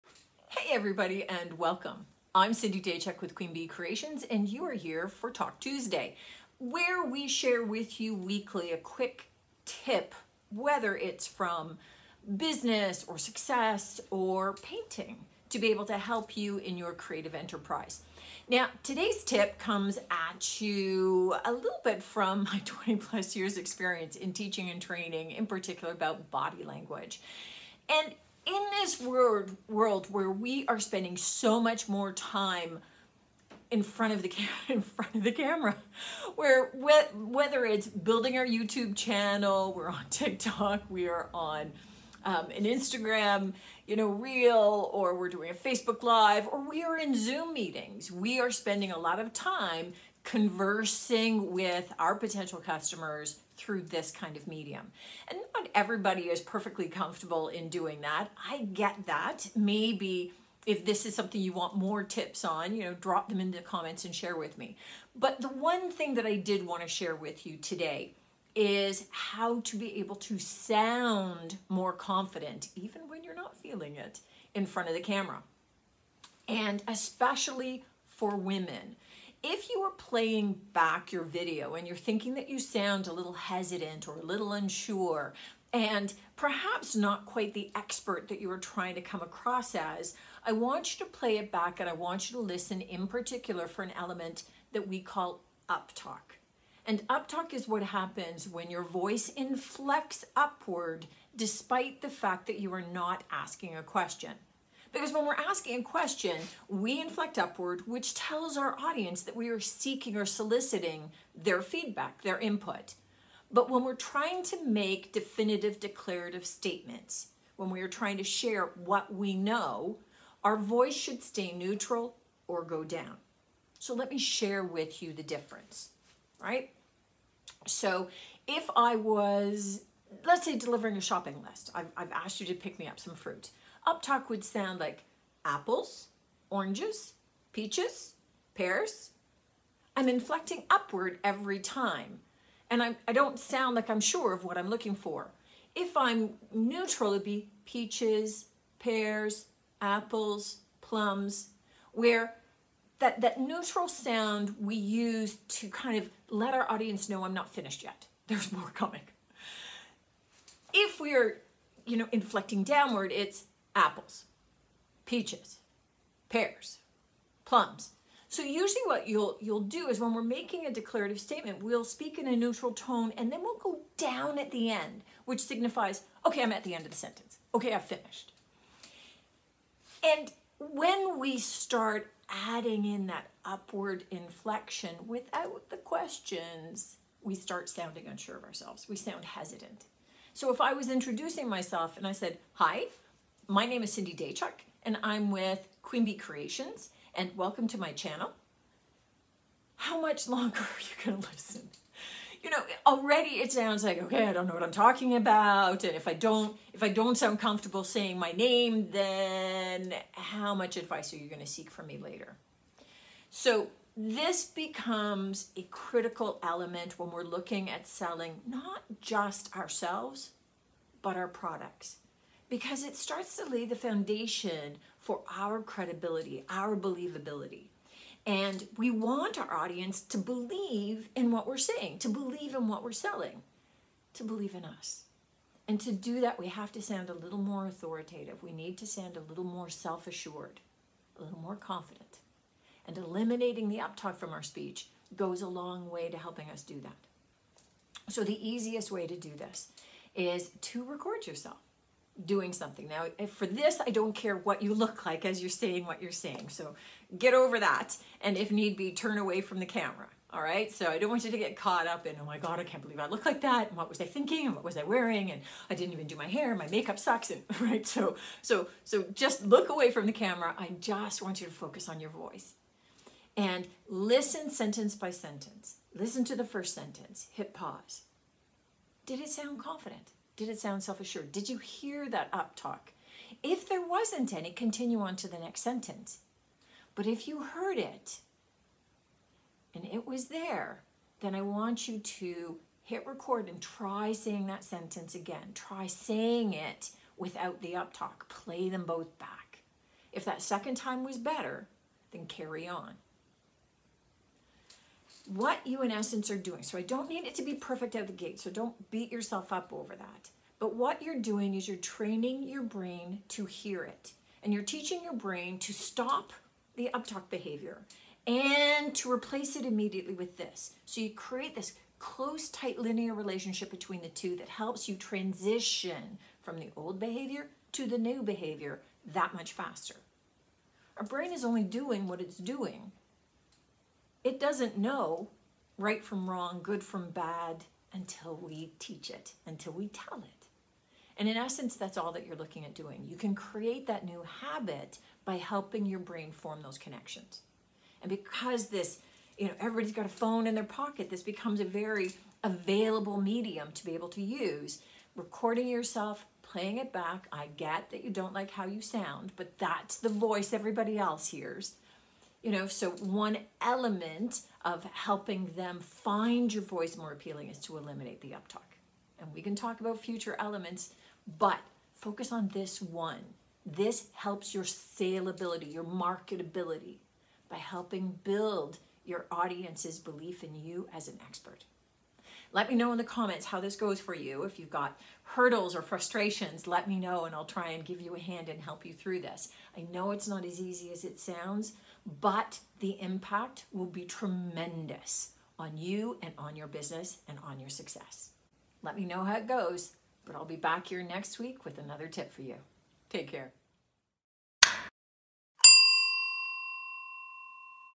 How to sound confident in videos
We may not always feel confident when we are putting ourselves out there on video, or delivering a speech, but learning to control your uptalk can help you sound more confident!  A quick tip you can work on that will leave you sounding more confident, even if you’re not feeling it!

or see what I’m up to creatively